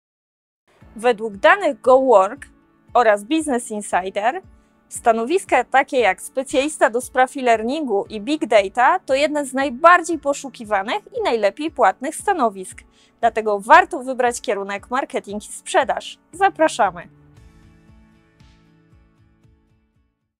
Marketing i sprzedaż na Uniwersytecie Dolnośląskim DSW
Dowiedz się, dlaczego warto wybrać kierunek marketing i sprzedaż na naszym uniwersytecie.